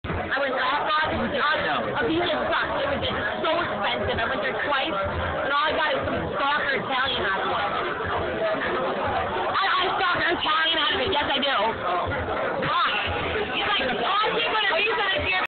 What You Get When You Visit Ibiza
Italian Stalkers Are The Courtesy Prize For Taking A Trip To Ibiza